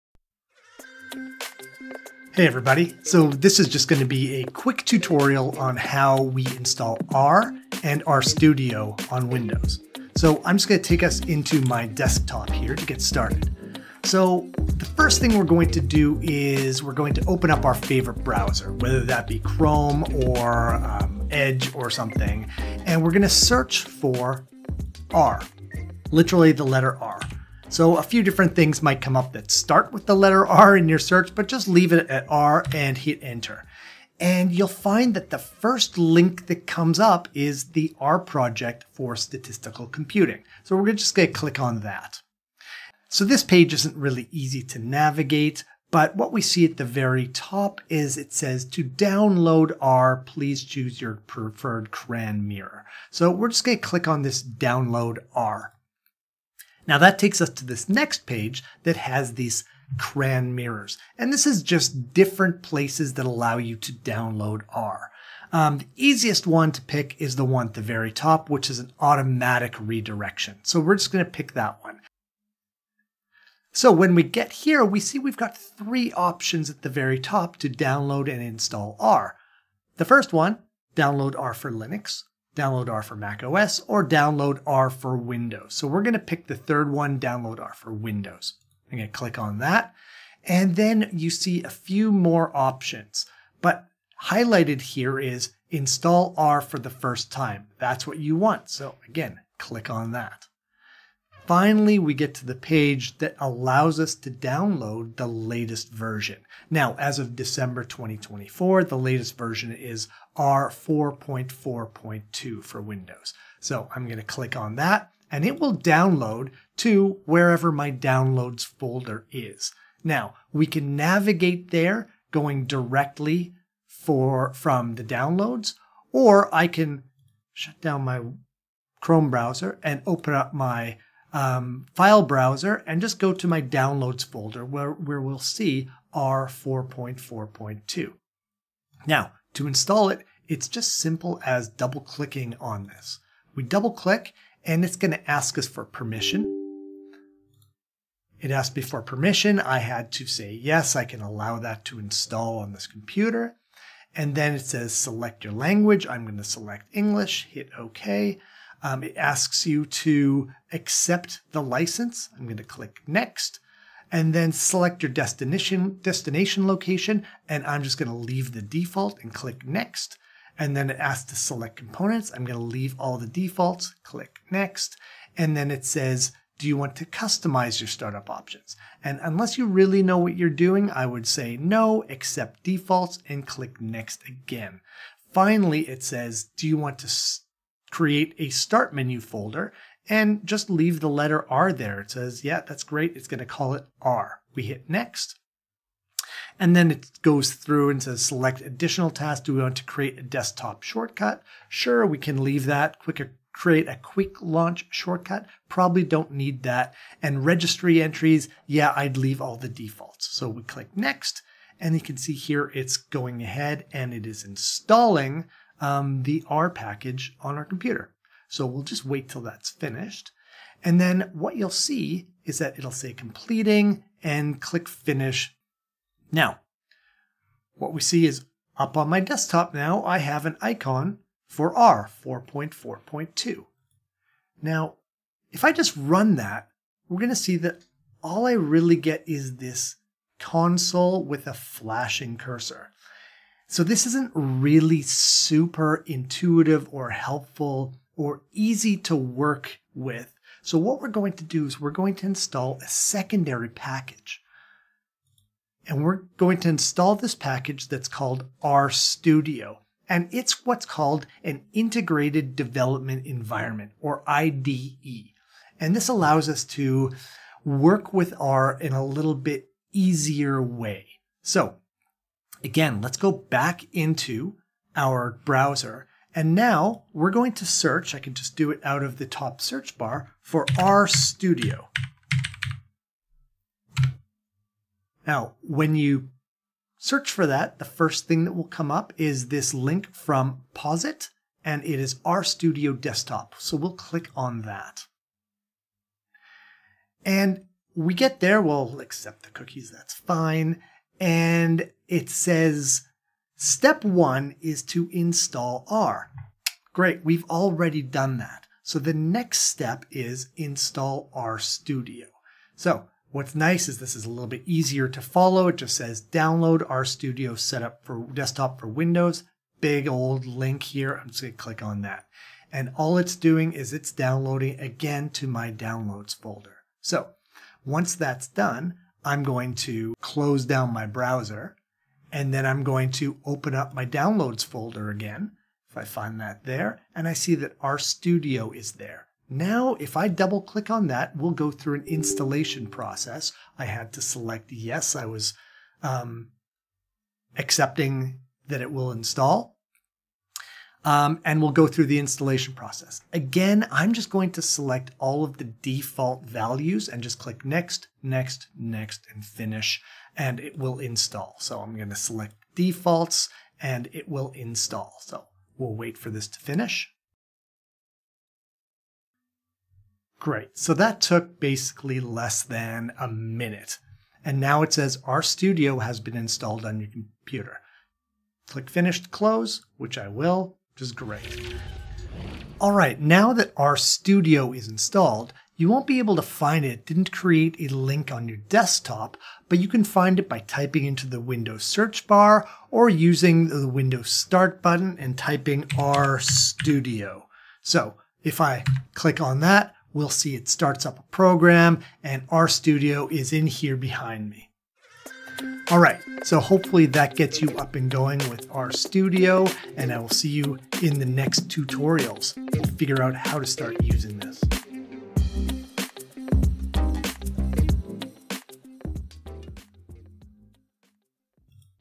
The Easiest way to install R and RStudio on Windows 11 (2025)
This video details the easy steps to download and install the R statistical package and the RStudio environment on Windows 11 in 2025. This video shows R version 4.4.2 and RStudio 2024.09